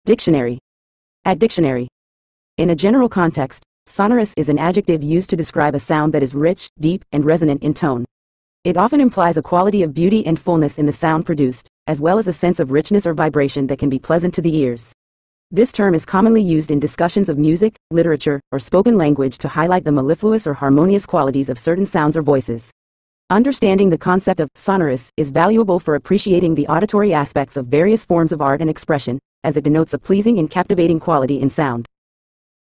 Sonorous Meaning In English
In a general context, "sonorous" is an adjective used to describe a sound that is rich, deep, and resonant in tone. It often implies a quality of beauty and fullness in the sound produced, as well as a sense of richness or vibration that can be pleasant to the ears. This term is commonly used in discussions of music, literature, or spoken language to highlight the mellifluous or harmonious qualities of certain sounds or voices. Understanding the concept of "sonorous" is valuable for appreciating the auditory aspects of various forms of art and expression, as it denotes a pleasing and captivating quality in sound.